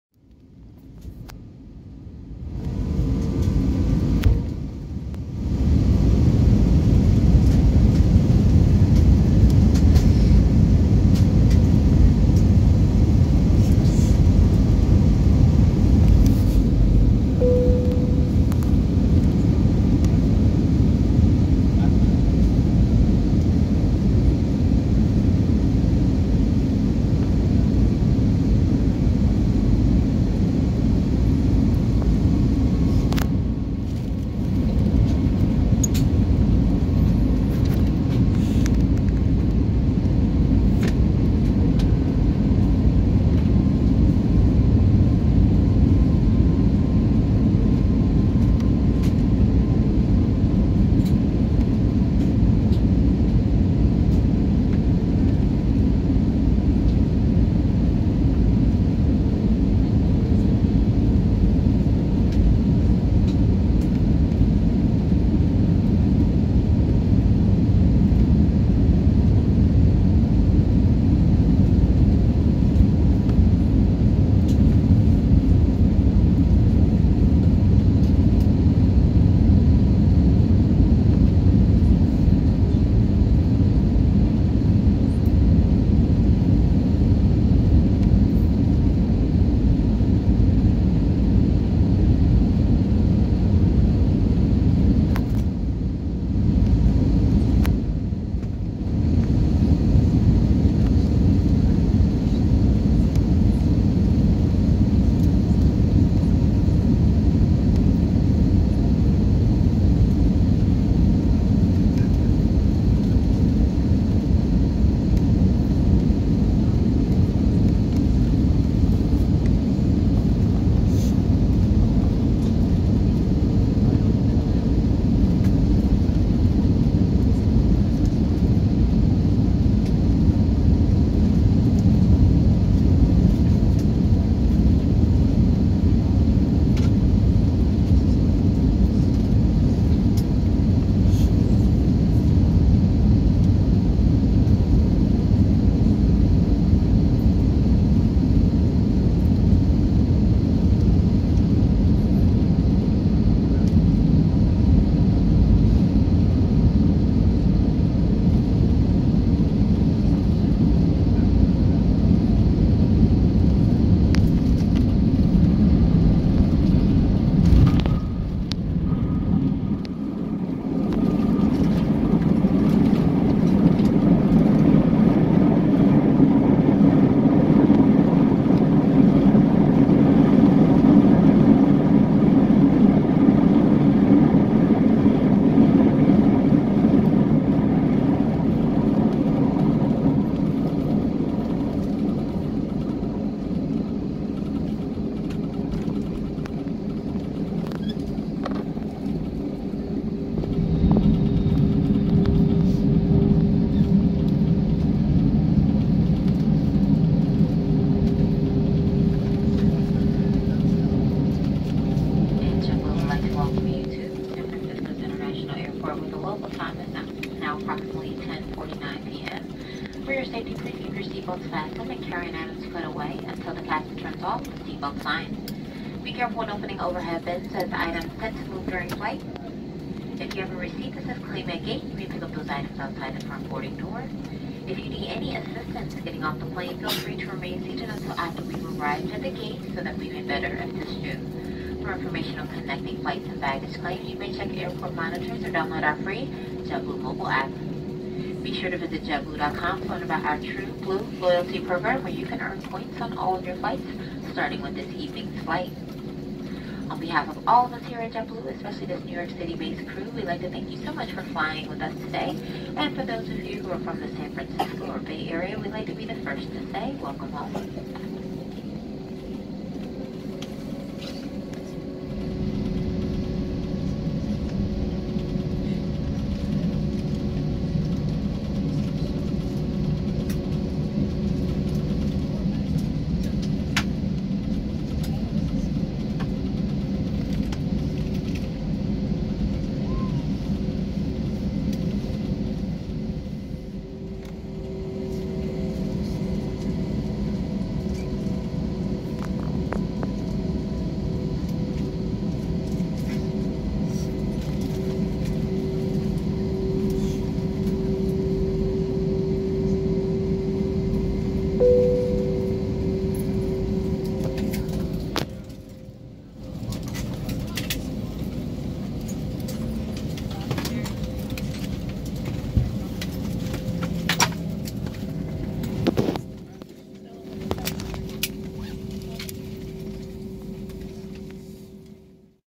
JetBlue Airbus A321 Landing at San Francisco (60fps)
Ah yes, more dark night-time recording which pays dividends for 60 frames-per-second video mode, which relies on more light getting entering the camera lens in the first place. I honestly really just prefer a high framerate that way though.

But at least we arrived almost an hour early which was enough to catch the last BART train from SFO.

Flight B6 1415 (JFK-SFO)
Airbus A321-231 (N943JT, Blue-Carpet TreatMint)
Recorded on Samsung Galaxy Note 8
Seat 24A (Core)